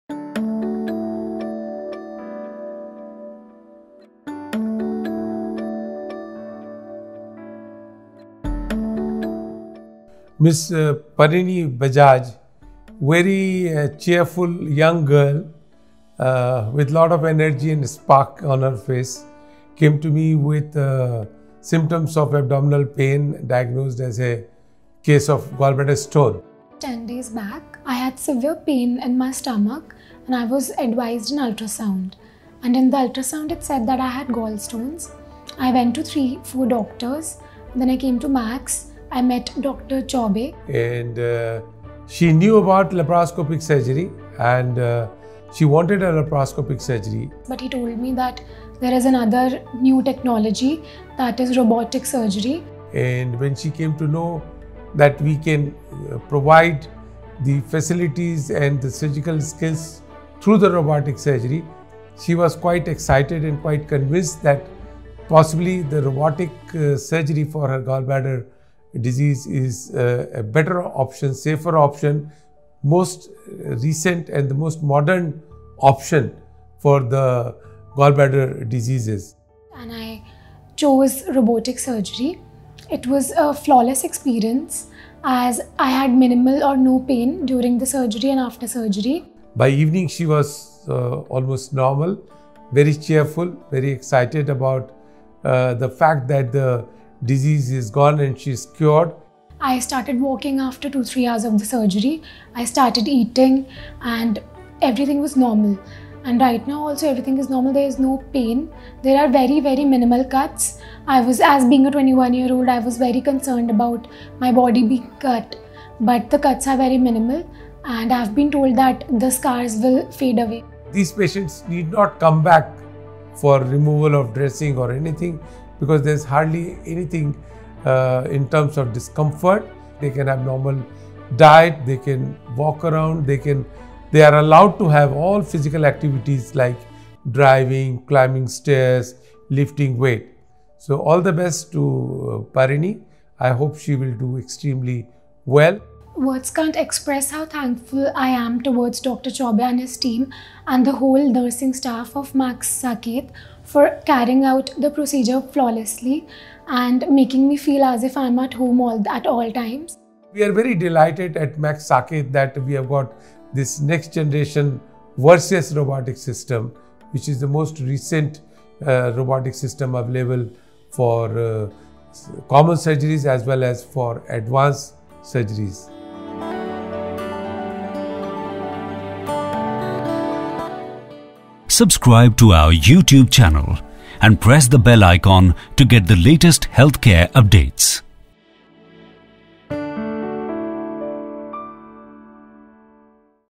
Robotic Surgery for Gallbladder Stone│Patient Success Story│Max Hospital, Saket
Ms. Parini Bajaj had intense pain in the abdominal region stemming from gallbladder stones. At the consultation with Dr. Pradeep Chowbey, Bajaj was convinced about the efficacy of the latest Robotic Surgery in removing the stones with minimal scarring and proceeded with the treatment.